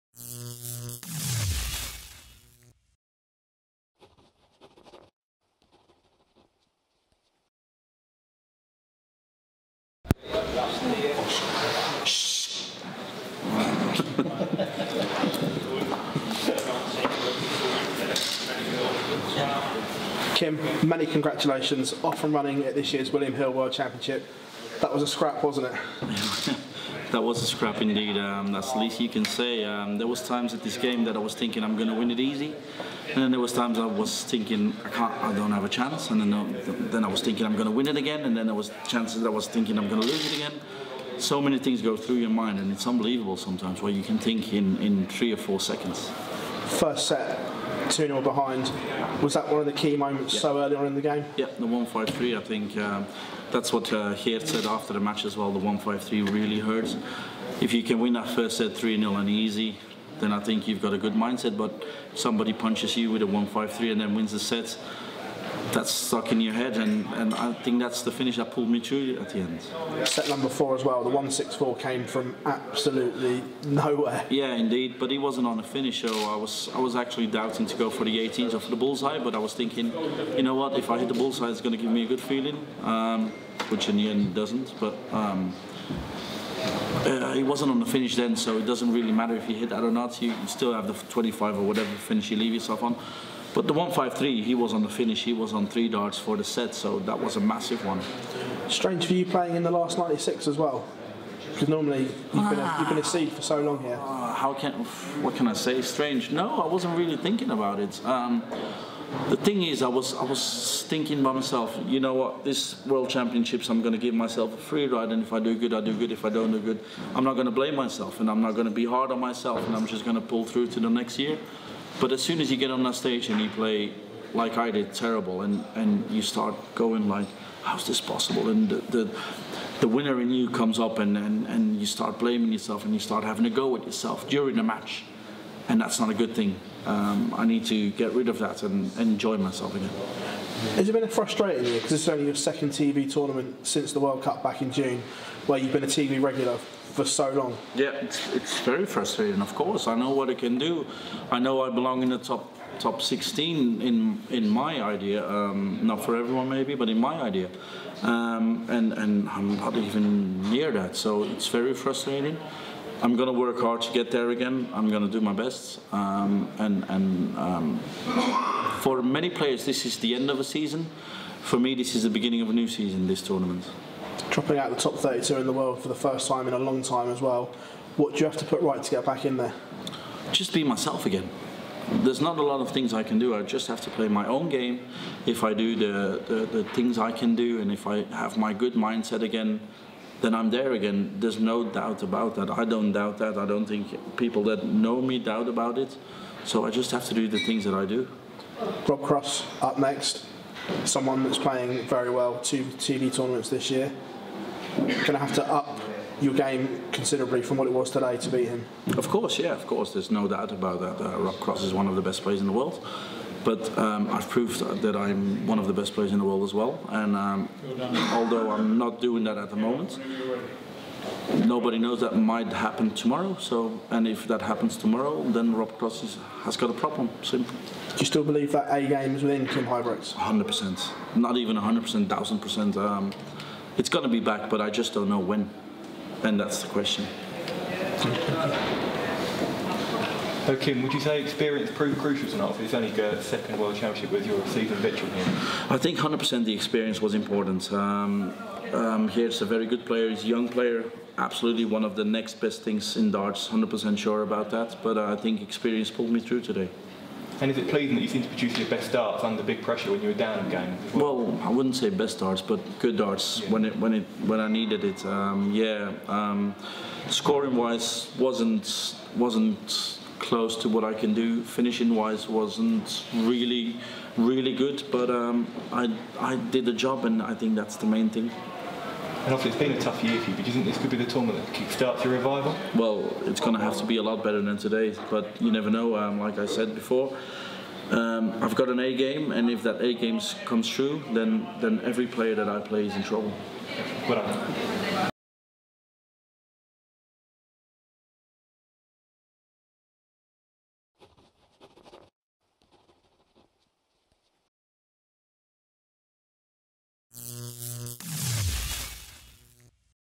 Kim Huybrechts: "My 'A' game is going to be back, 100 per cent, I just don't know when"
Former major finalist Kim Huybrechts reflects on his hard-fought deciding set win over Dutch youngster Geert Nentjes in Round One of the William Hill World Championship.